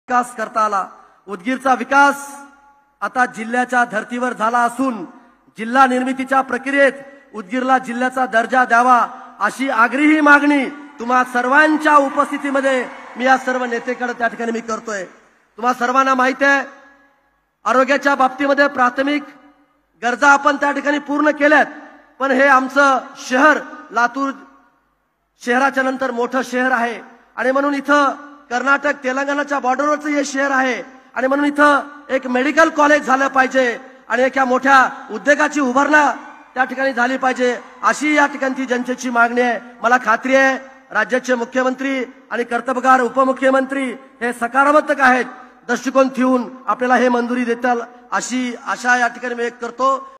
उदगीर जिल्हा करावायासाठी नामदार संजय बनसोडे यांचे मुख्यमंत्री उपमुख्यमंत्री यांना साकडे
उदगीर चा विकास जिल्ह्याच्या धरतीवर झाला असून आता उदगीरला जिल्हा जाहीर करावा व उदगीर शहरात एक मेडिकल कॉलेज व मोठा उद्योग निर्मिती केंद्र स्थापित करावे अशी मागणी नामदार संजय बनसोडे यांनीराष्ट्रपतींच्या उपस्थितीमध्ये मुख्यमंत्री ,उपमुख्यमंत्रीयांच्याकडे केली .